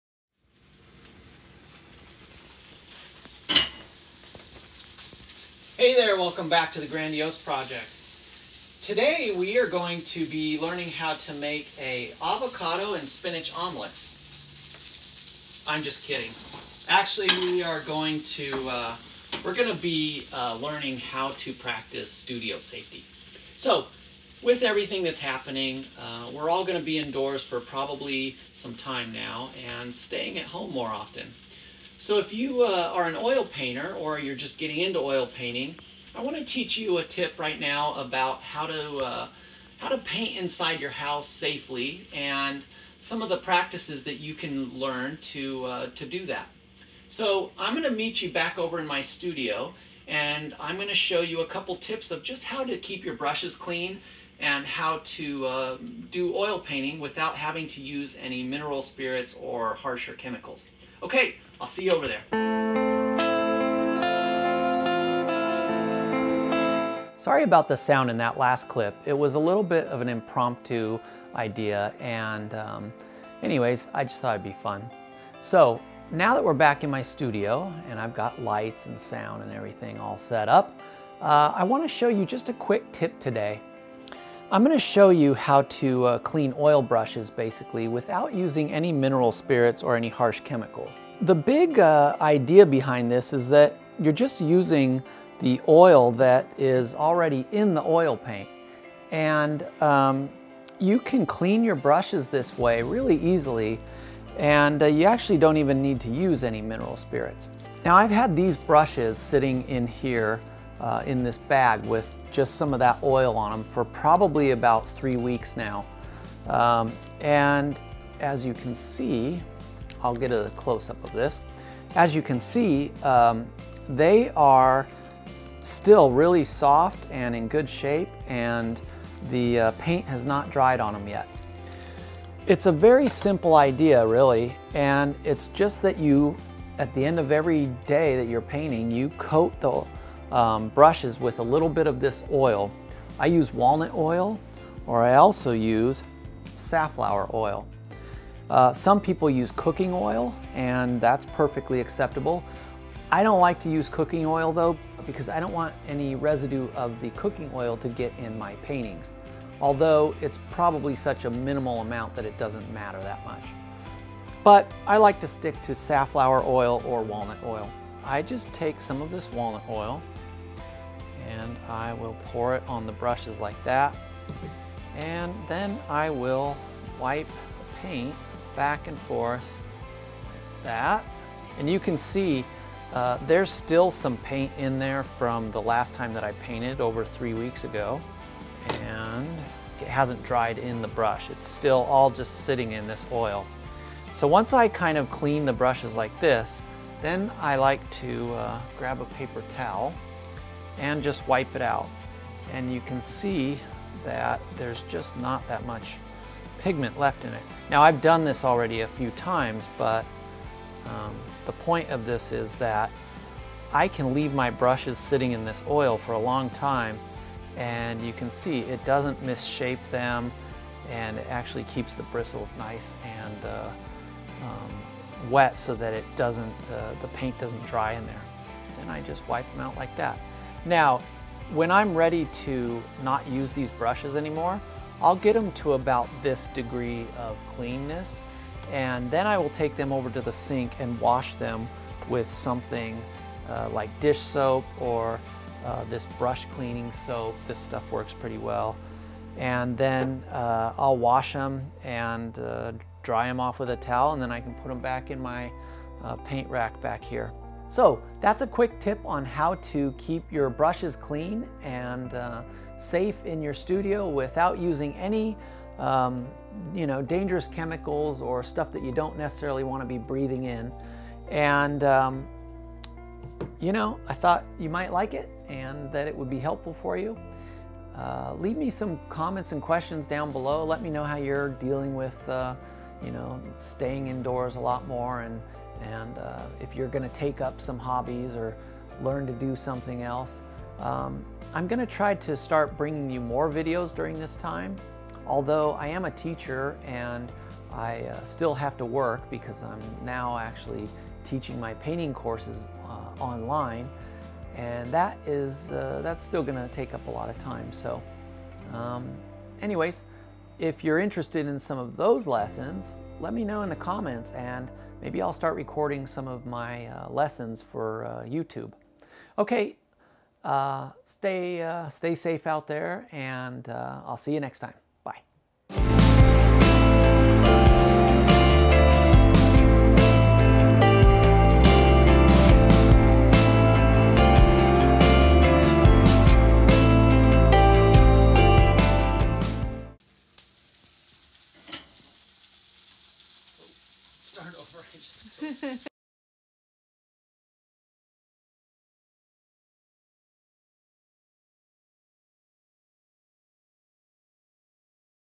Studio Safety | Cleaning Your Oil Painting Brushes | How To!
Studio Safety - Cleaning Your Oil Painting Brushes! Learn how to clean your oil painting brushes the safe way, without using mineral spirits or other harsh chemicals! Take the natural approach of using harmless oil to eliminate mineral spirits or turpentine from your studio. In this weeks episode I show you this quick tip of how I cleaned up the air in my studio and made it a much safer place to work.